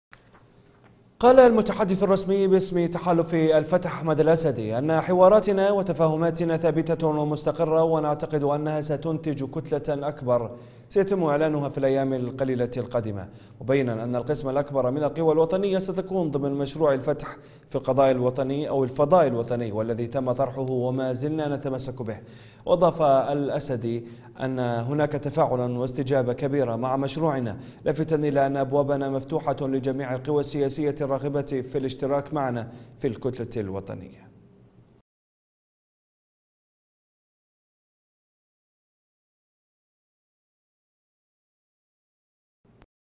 احمد الاسدي: تفاهماتنا ثابتة وستنتج كتلة أكبر سيتم إعلانها في الايام القليلة القادمة
المتحدث الرسمي باسم تحالف الفتح
وتفاهماتنا ثابتة وستنتج كتلة أكبر سيتم إعلانها في الايام القليلة القادمة

قال المتحدث الرسمي باسم تحالف الفتح أحمد الأسدي، إن حواراتنا وتفاهماتنا ثابتة ومستقرة ونعتقد أنها ستنتج كتلة أكبر سيتم إعلانها في الايام القليلة القادمة ، مبيناً أن القسم الأكبر من القوى الوطنية ستكون ضمن مشروع الفتح في الفضاء الوطني والذي تم طرحه ومازلنا نتمسك به . وأضاف الأسدي، إن هناك تفاعلاً واستجابة كبيرة مع مشروعنا ، لافتاً إلى أن أبوابنا مفتوحة لجميع القوى السياسية الراغبة في الاشتراك معنا في الكتلة الوطنية .